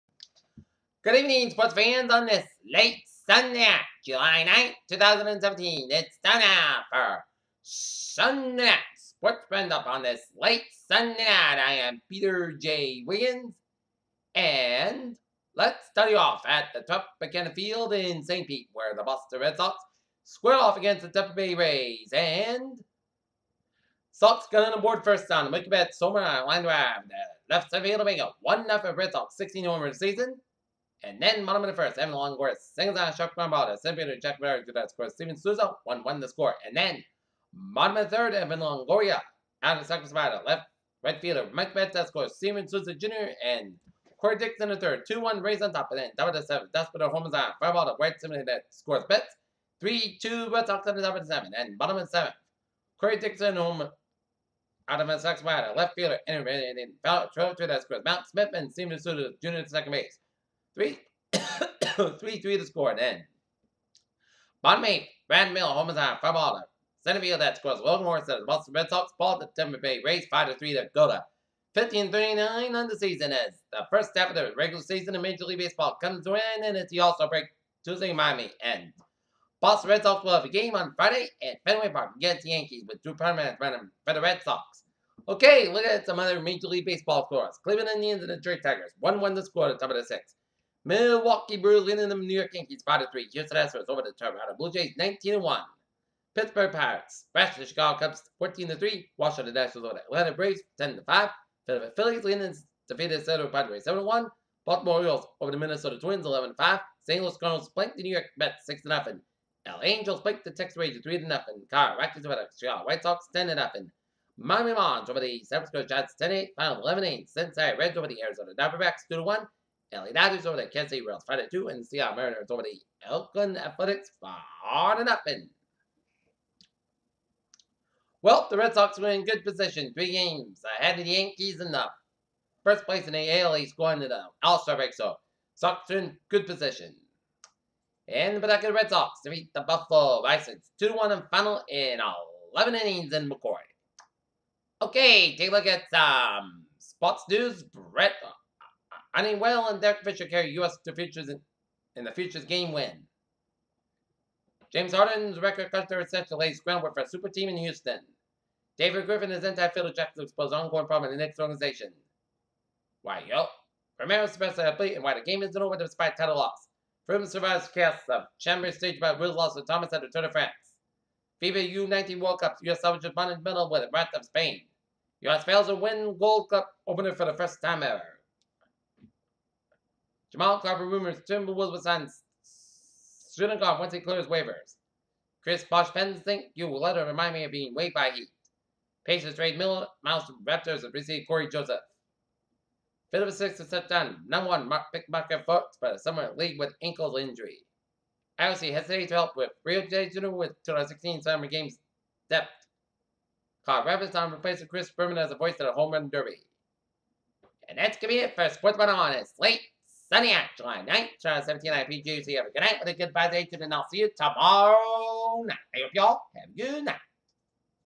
Sunday Night Sports Roundup with Peter J. Wiggins- Sunday July 9, 2017
A summary of tonight's sports action

1. Boston Red Sox lose to the Tampa Bay Rays at Tropicana Field, 5 to 3 and a look at the MLB scoreboard

2. Pawtucket Red Sox defeat the Buffalo Bisons at McCoy Stadium,  2 to 1 in 11 innings

3. Sports News